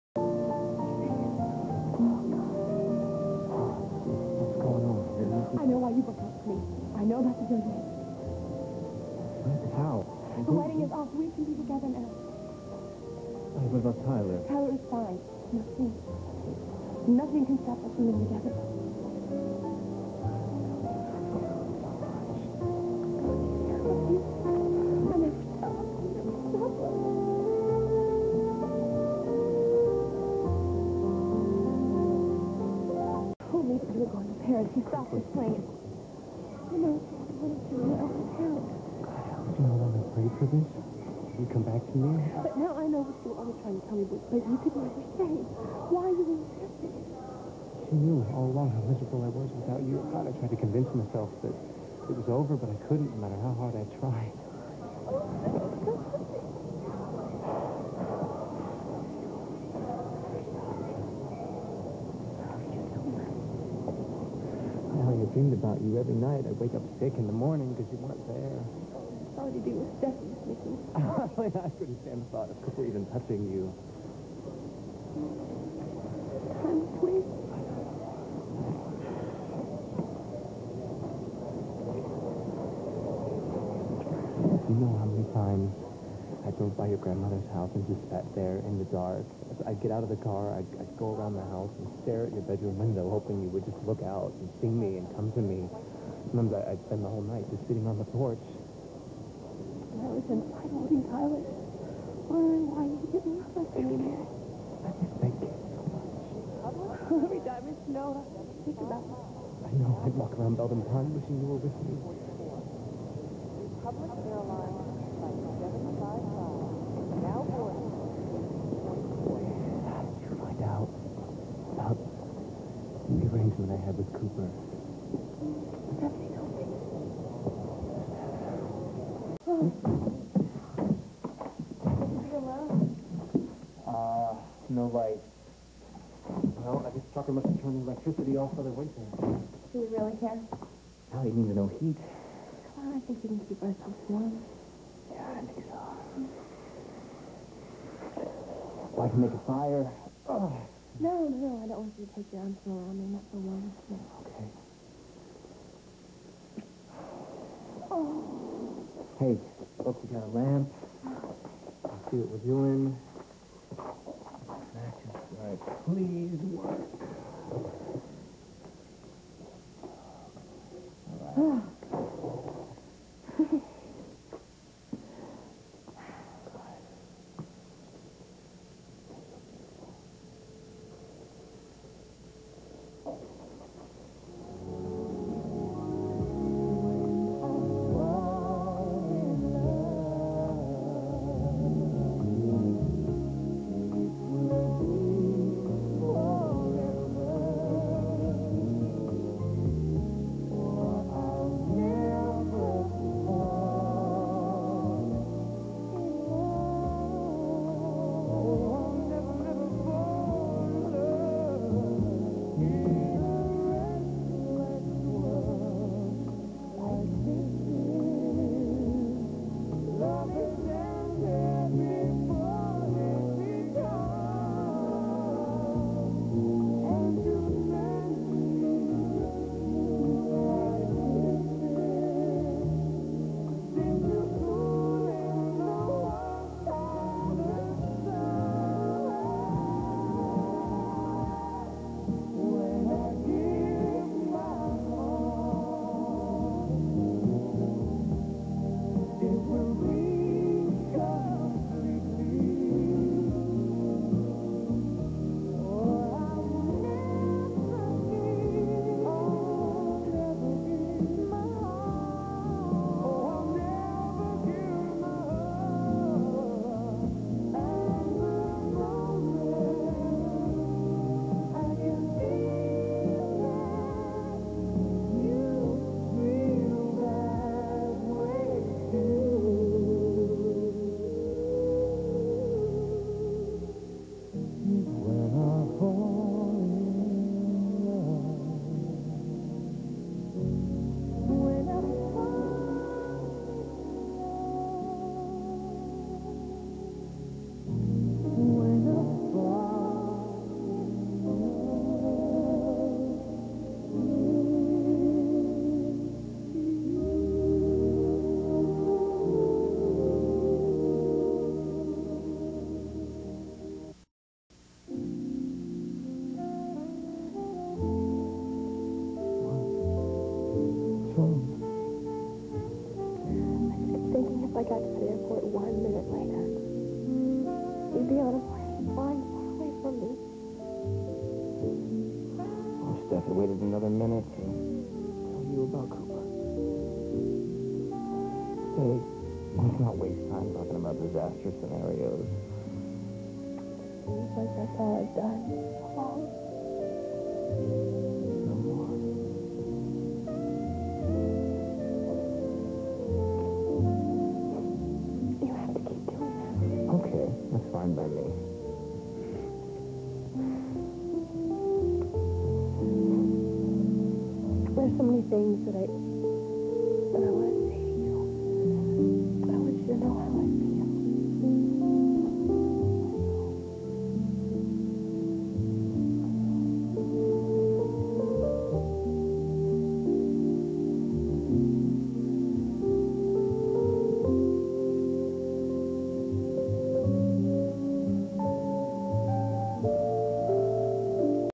Loving - Casey
Ally and Casey reconcile and make love for the second time at Trucker's cabin.